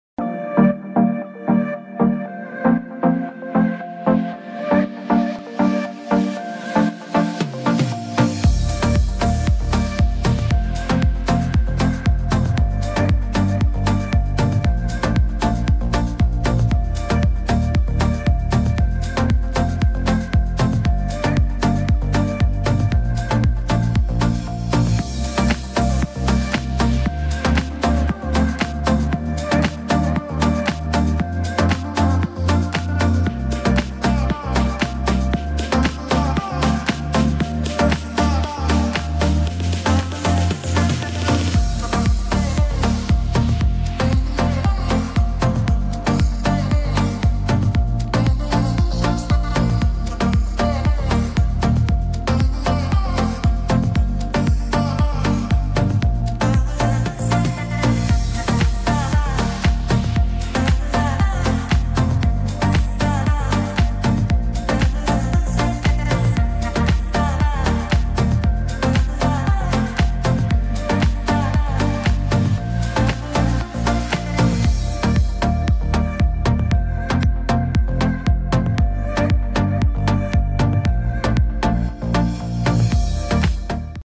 امرأة ترأس منظمة إسلامية ! 🧕🏻🇬🇧
زارا محمد أول امرأة تتولى إدارة منظمة إسلامية كبيرة في بريطانيا ! 🧕🏻🇬🇧